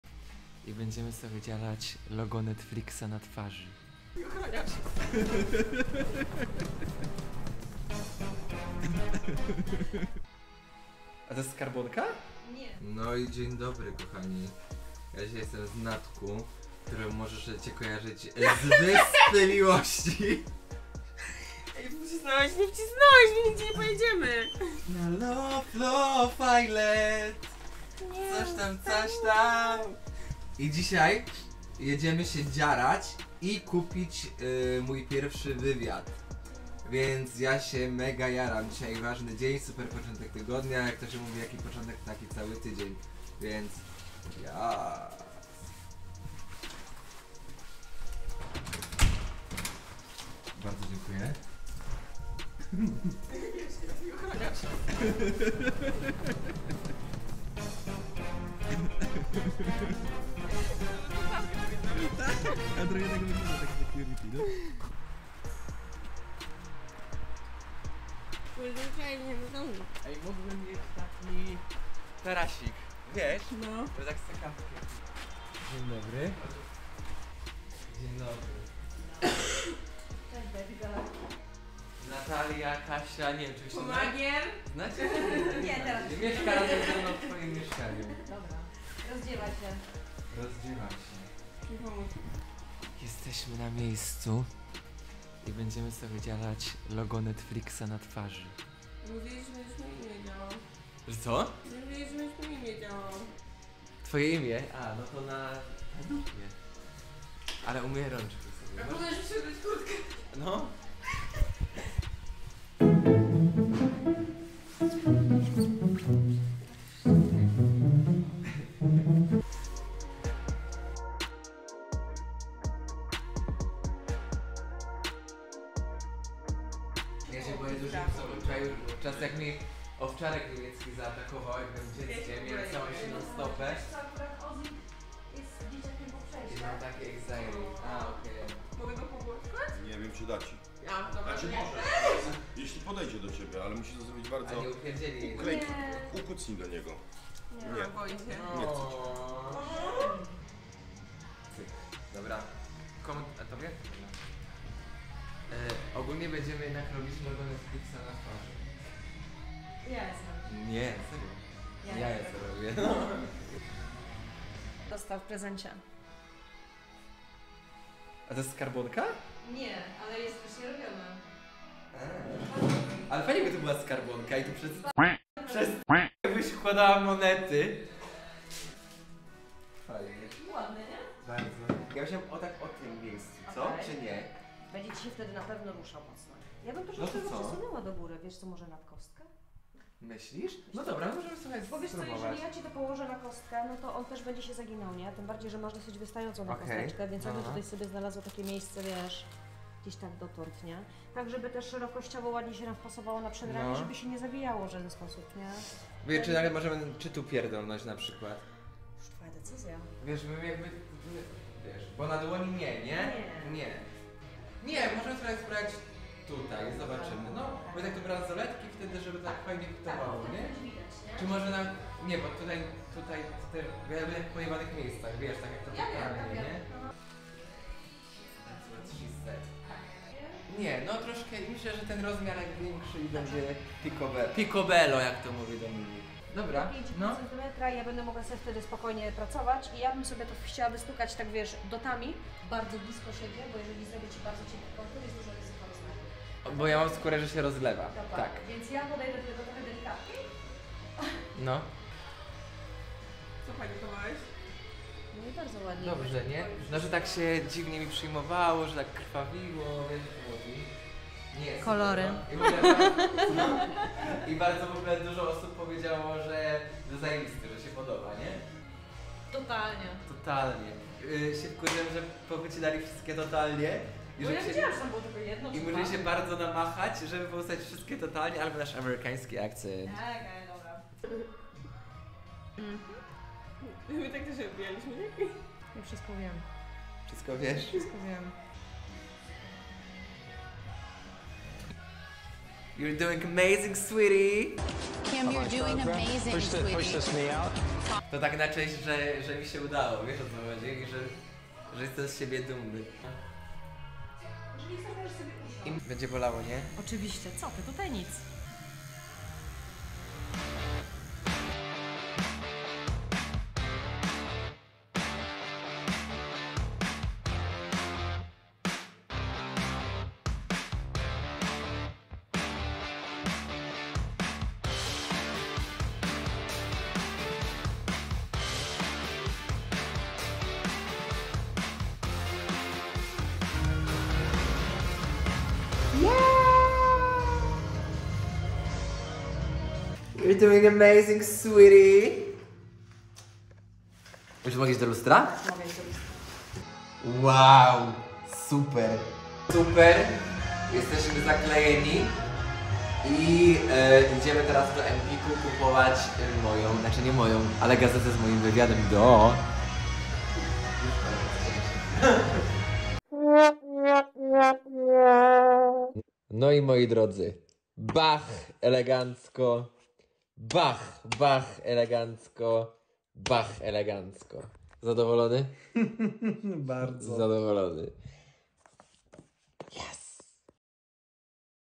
VLOG #2 TATUUJE SOBIE LOGO NETFLIXA NA TWARZY?!
CZEŚĆ!

Zabieram was ze sobą na dziarę! Z okazji premiery programu Love Never Lies Polska, postanowiłem zrobić sobie pamiątkowy tatuaż. Serio postanowiłem wydzierać sobie twarz?